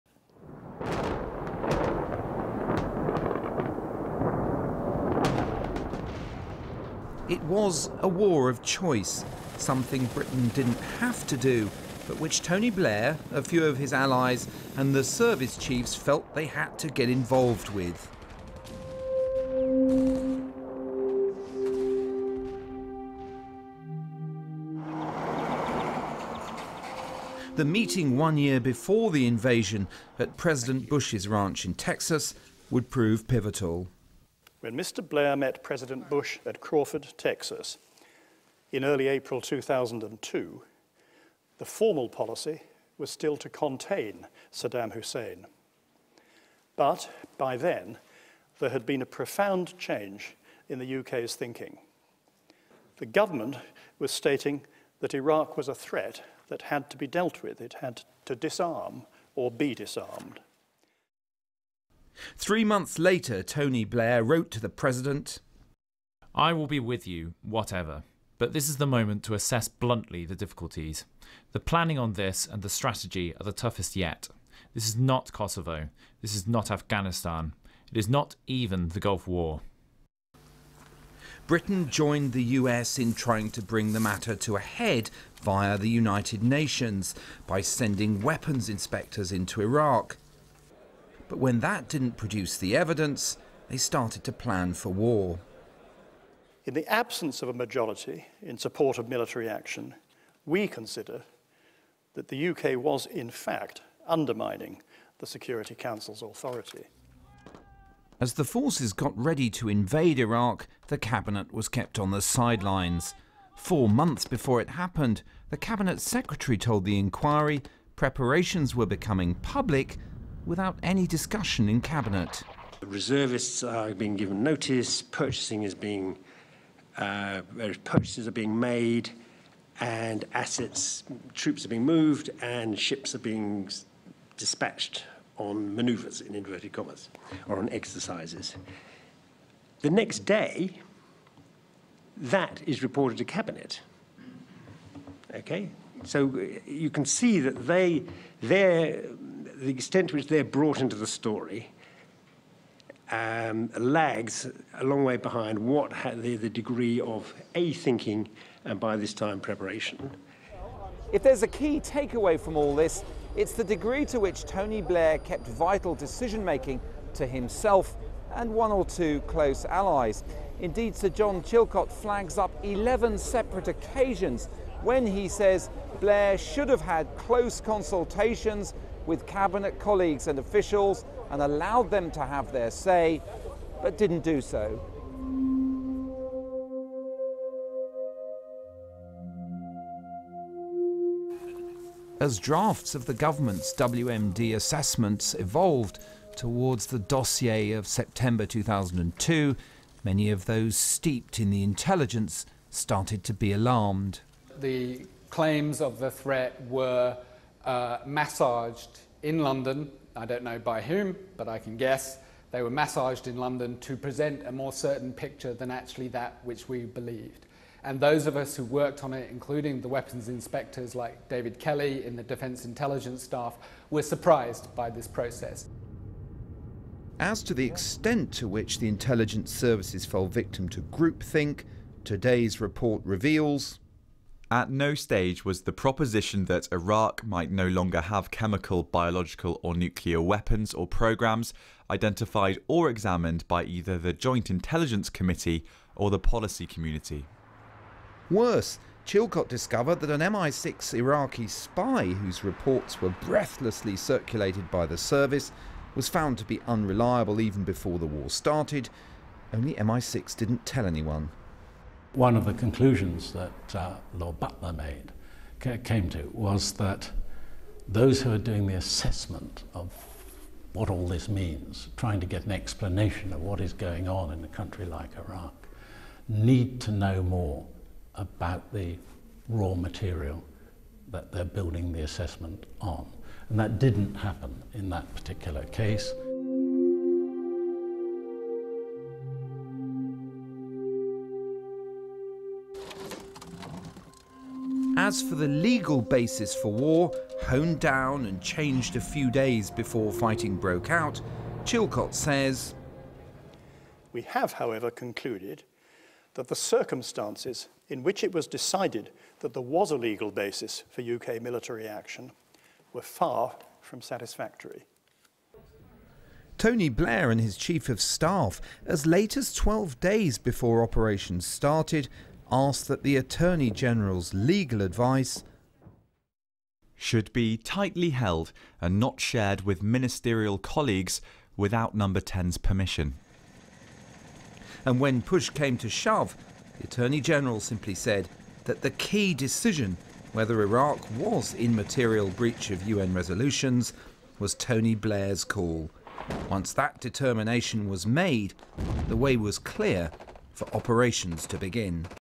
Iraq War - How We Got There
Diplomatic editor, Mark Urban looks at the build up to the Iraq War